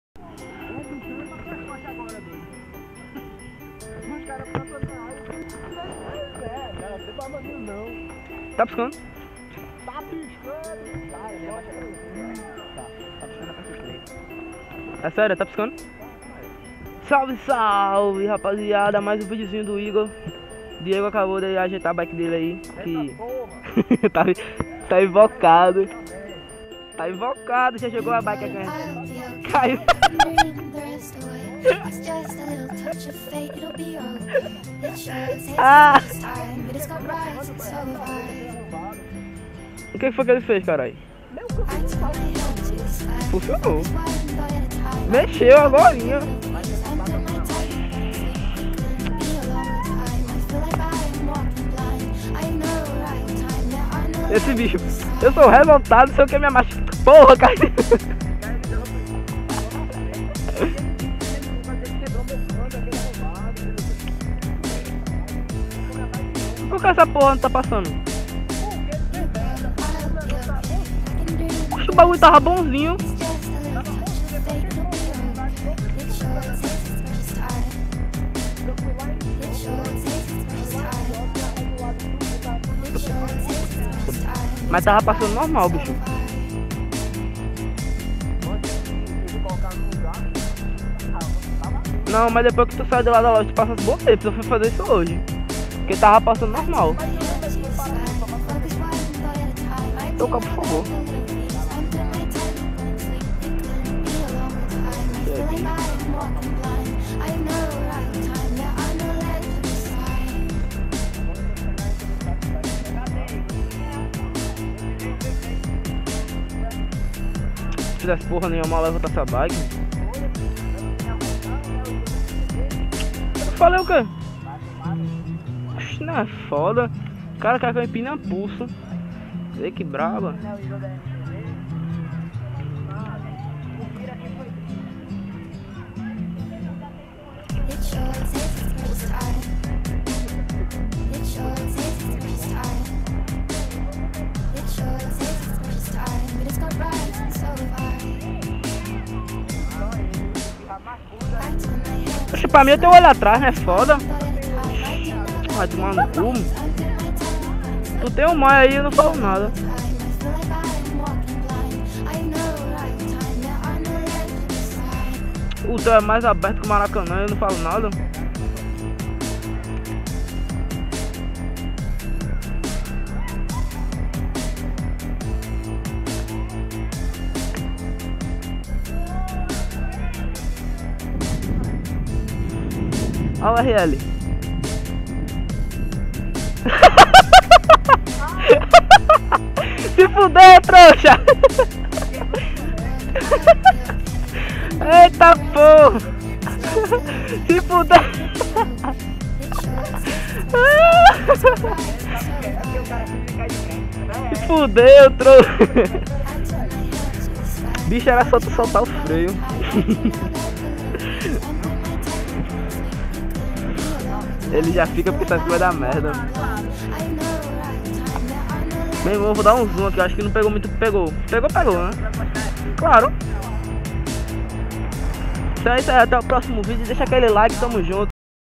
VIREI NO BABALÚ "Rl" - Rolê Top - Testando A Posição Da Camera - IGOR DA MTB
VIREI NO RL ROLE TOP NA JAQUEIRA TESNTADO A POSICAO DA CAMERA SJCAM E MICROFONE BY TONH MILLER

E Ai Pessoal ? Beleza ? Mais Um Vídeo Do Igor Da Mtb Mlk !! Toma Grau Tio Haa ! É Nois Familiaa !! Vcs São Foda !

✸ Canais Que Eu SUPER Recomendo! ✸